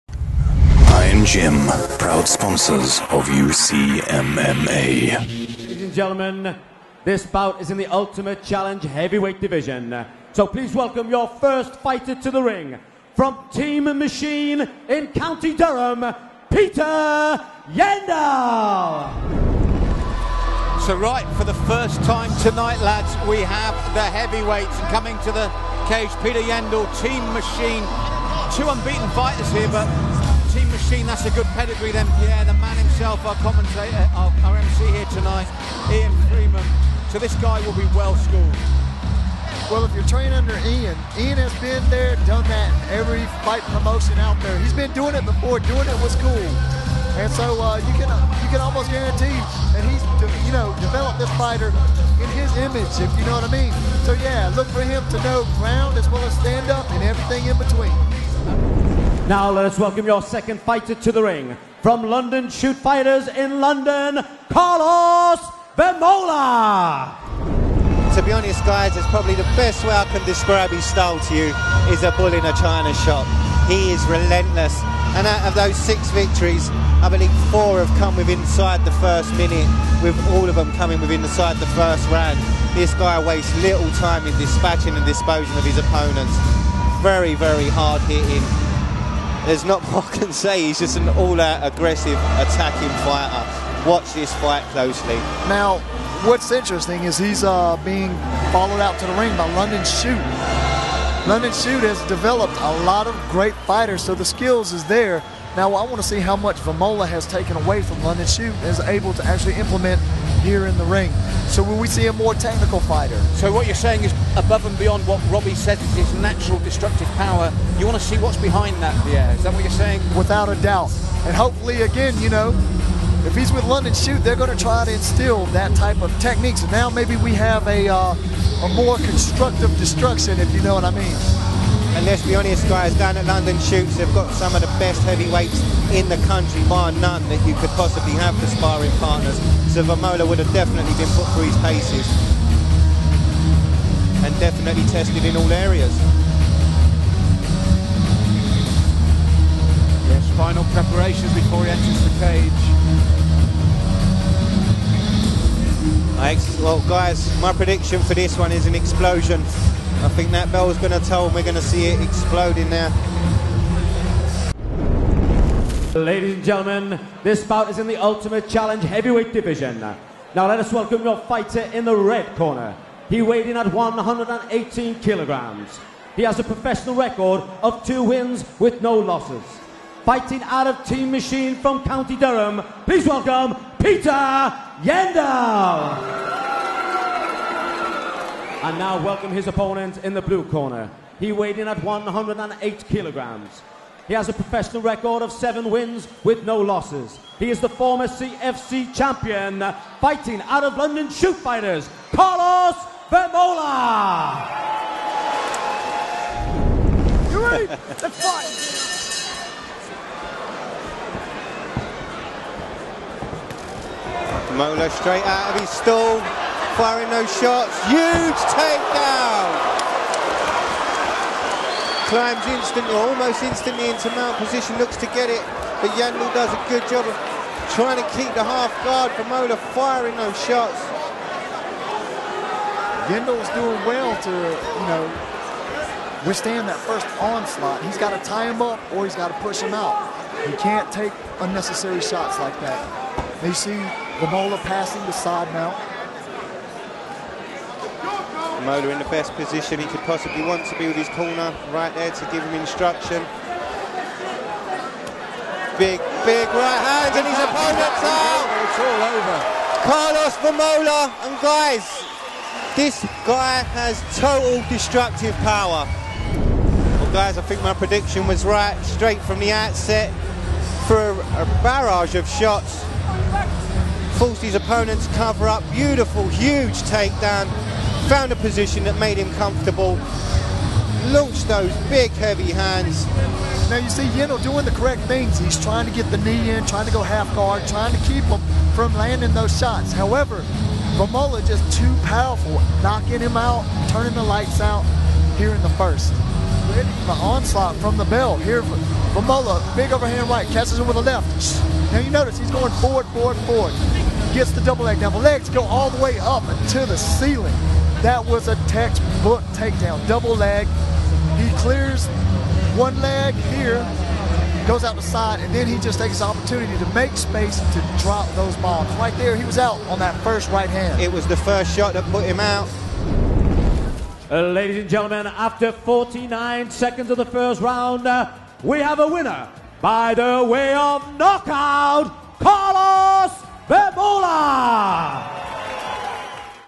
UCMMA 10: Peter Yendall Vs Karlos Vemola
A Video from the UCMMA vault, featuring UFC Star Karlos Vemola challenged by Peter Yendall. Both fighters went into this fight with clean records, some ones '0' has to go.